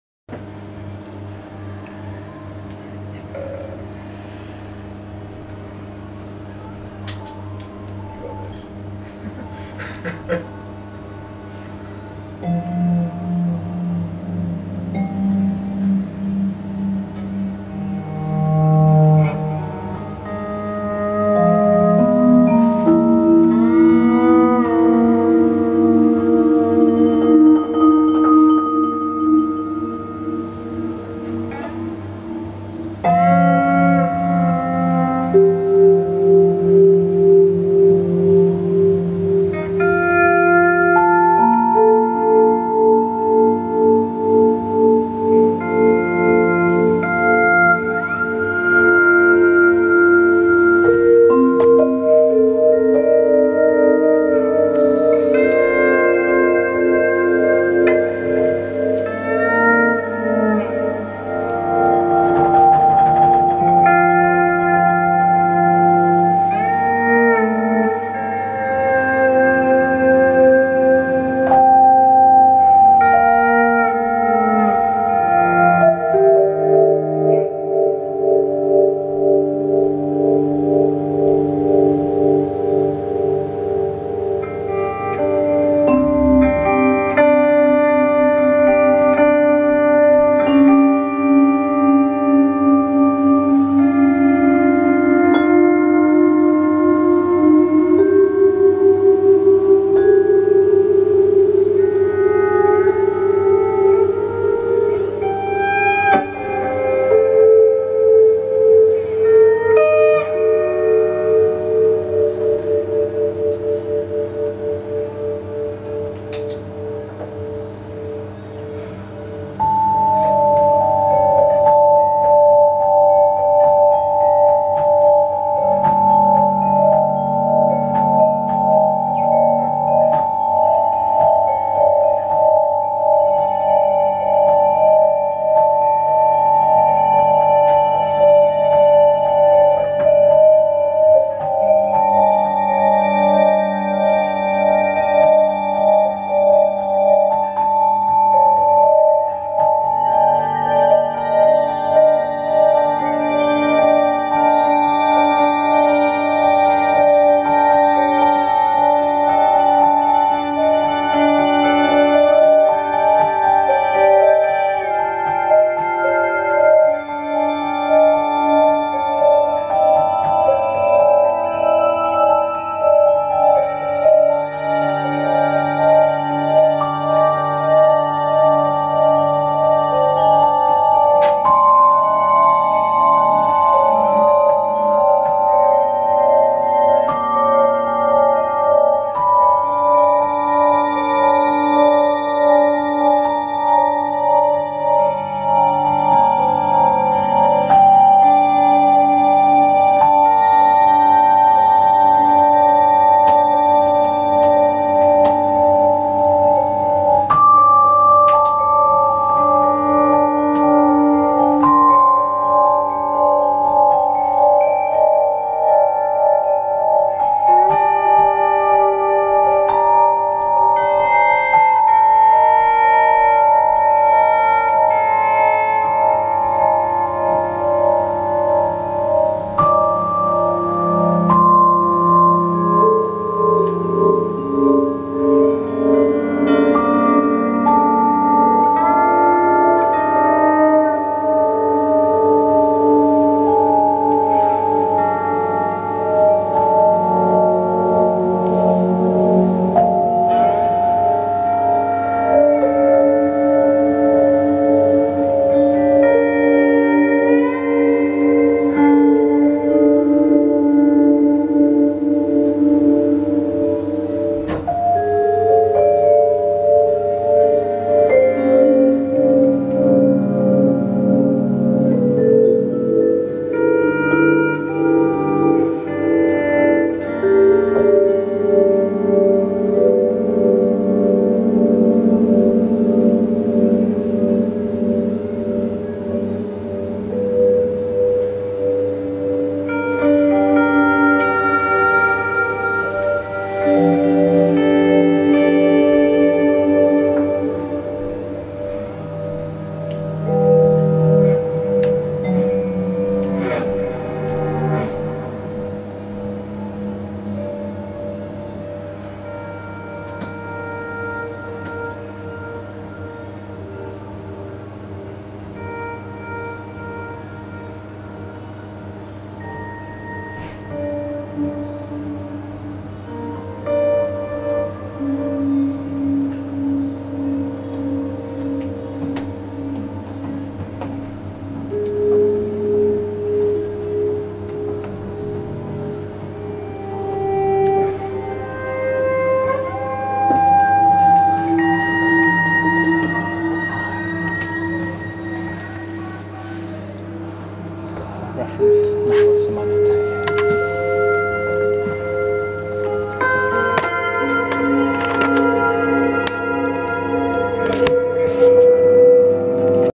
Adam Bushell and Tim Sheinman
Adam Bushell - Vibes
Tim Sheinman - pedal Steel
Live at the Rossi Bar, Brighton 18th June 2019